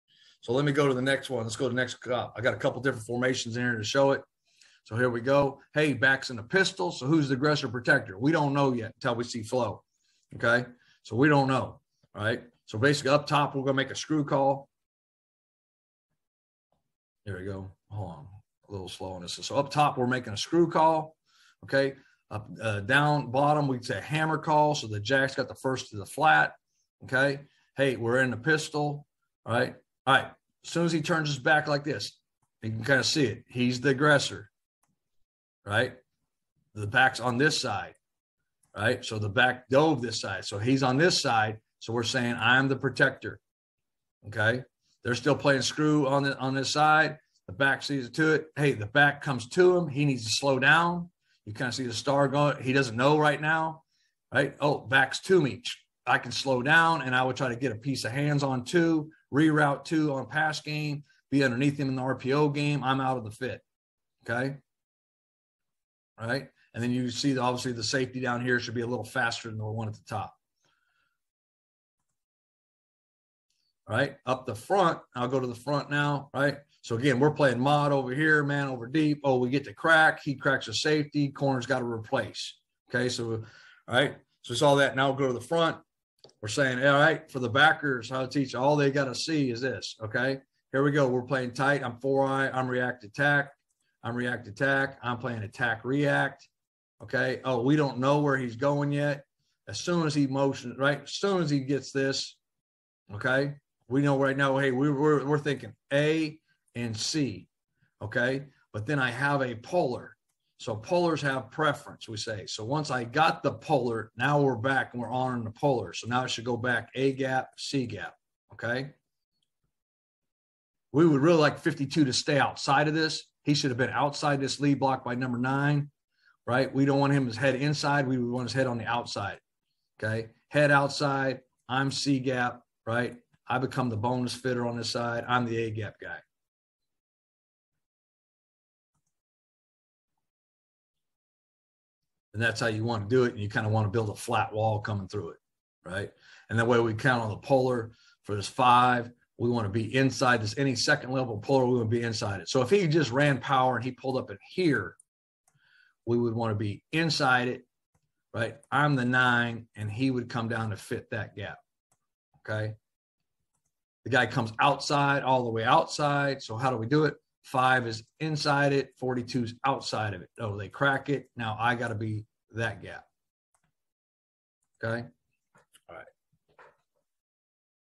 Ron Roberts - Run Fits vs Pistol
Baylor DC Ron Roberts talking about how they manipulate their run fits vs Pistol based on the side of the back path post snap.